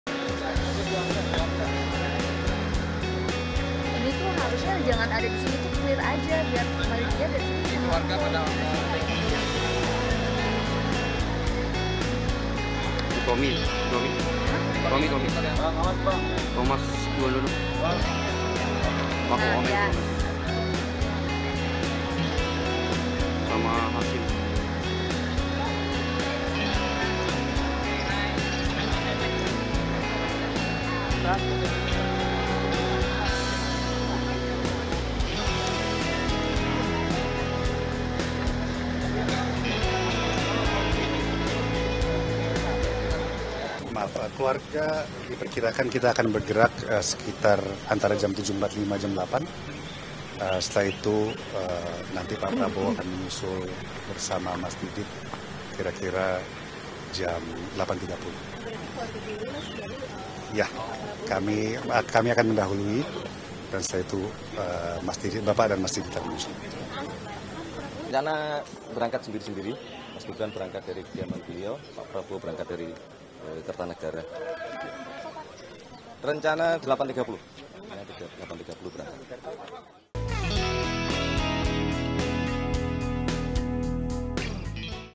Keluarga dan Kerabat Hadir Jelang Pelantikan Prabowo
Sejumlah anggota keluarga mulai berdatangan di kediaman pribadi Prabowo Subianto di jalan Kertanegara 4, Jakarta Selatan. Kerabat yang hadir di antaranya adik Prabowo, Hashim Djojohadikusumo,  Thomas Djiwadono, Budi Djiwandono, hingga Prasetyo Hadi.

Nantinya Prabowo akan didampingi keluarga menuju gedung DPR MPR RI.